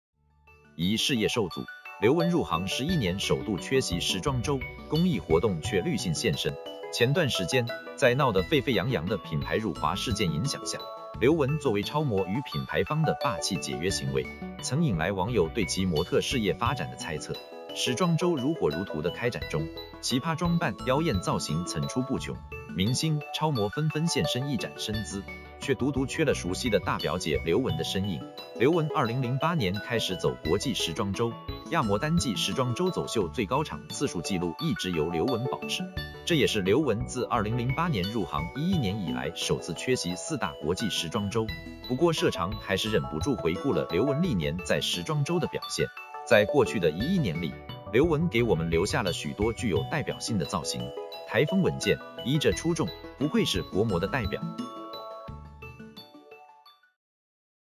疑事业受阻？刘雯入行11年首度缺席时装周,公益活动却率性现身
➤Thanks收看！